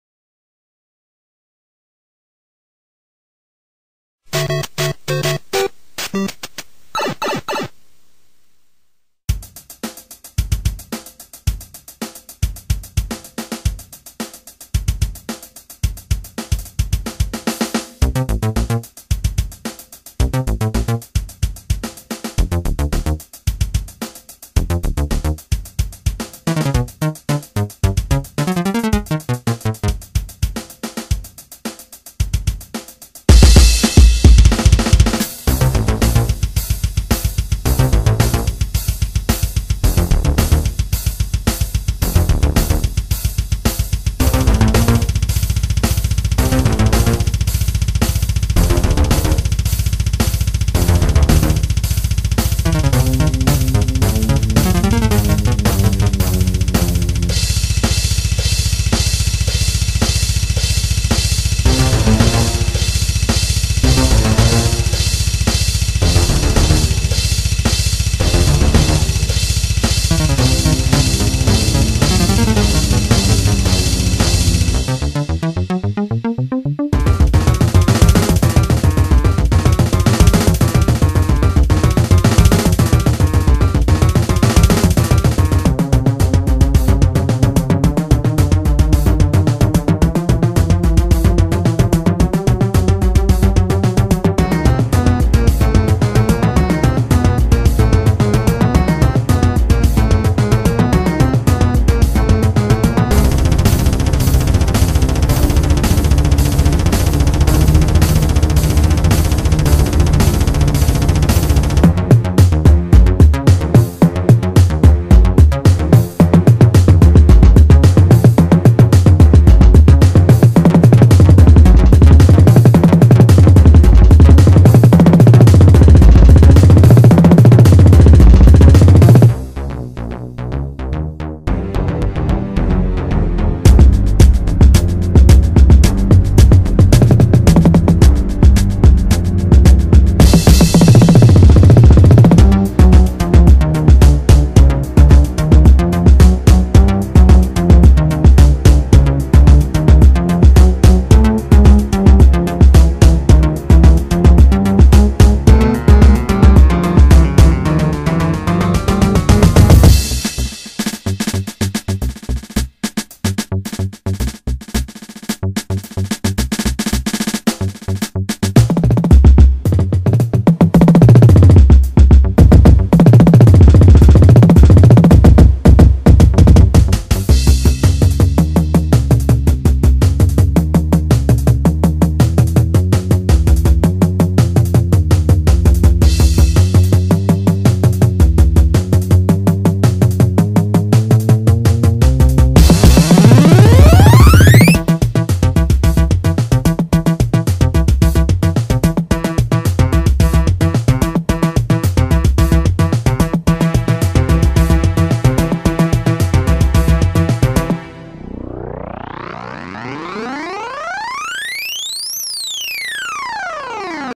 Mario Extreme
The extreme verison of Marios quest to save the princess...DRUM BATTLE INCLUDED!